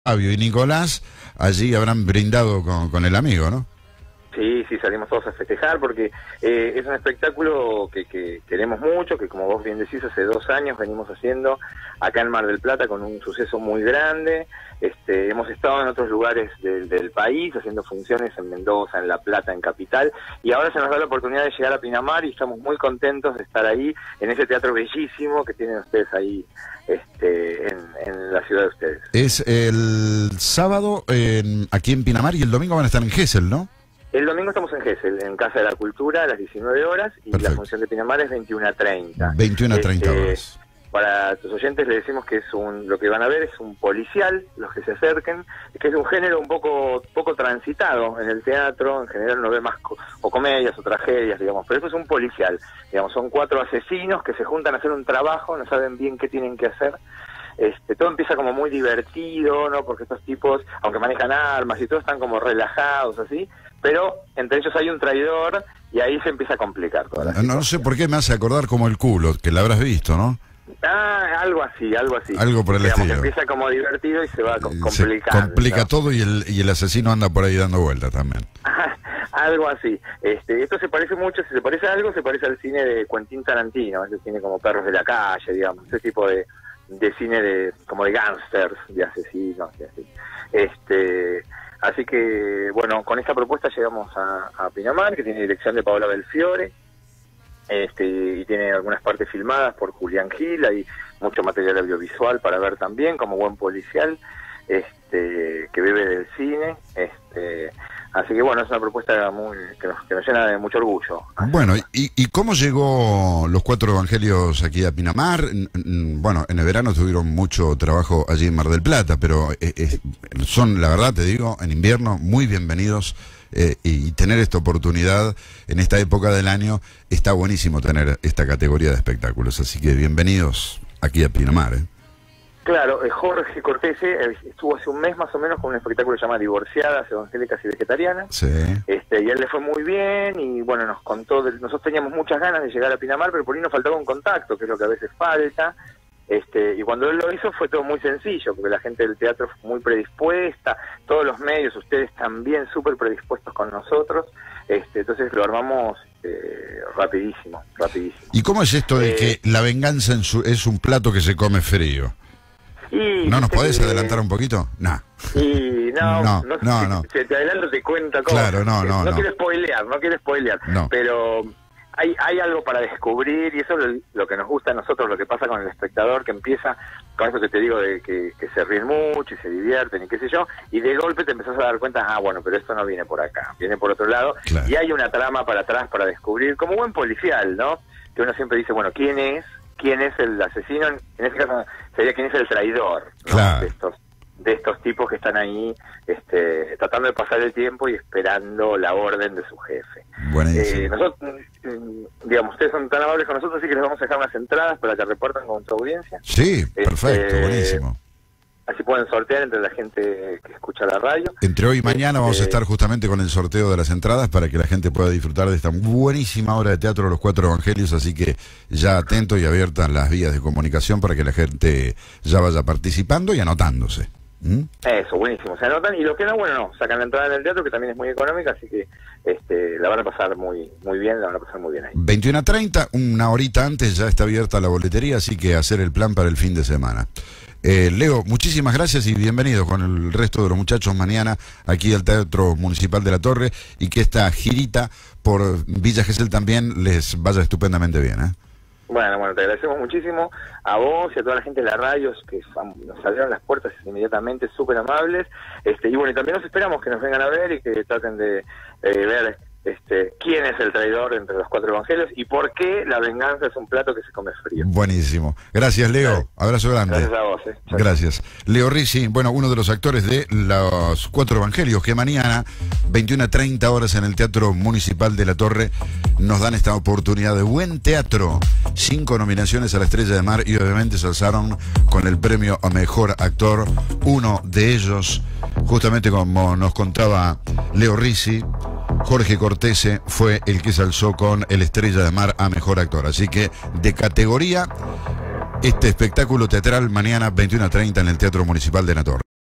LOS 4 EVANGELIOS - LEO RIZZI (ACTOR) MAÑANA
LOS 4 EVANGELIOS - LEO RIZZI (ACTOR) MAÑANA SABADO 21 HS TEATRO DE LA TORRE | Para mas detalles ver